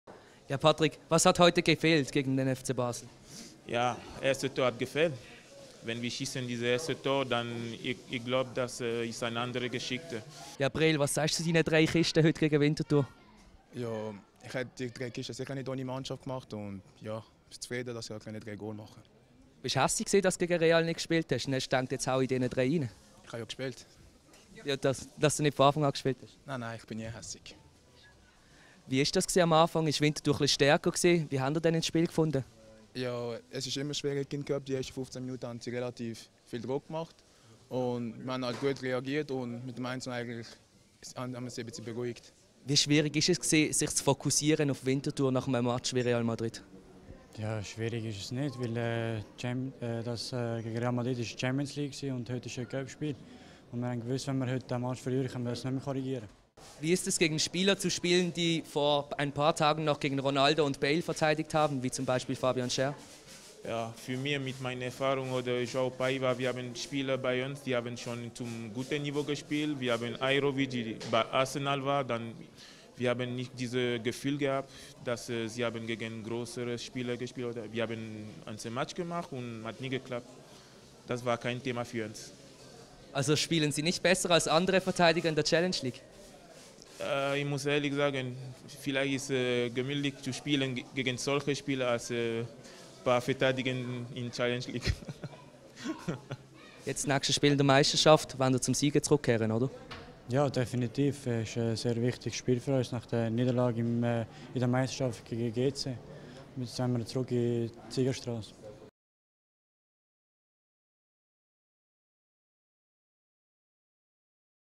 Bengondo: «Gegen Schär ist es gemütlicher als in...
Das sagen FCB-Juwel Embolo, Naser Aliji und Patrick Bengondo zum Cup-Spiel Winti-FCB.

Sport,Fussball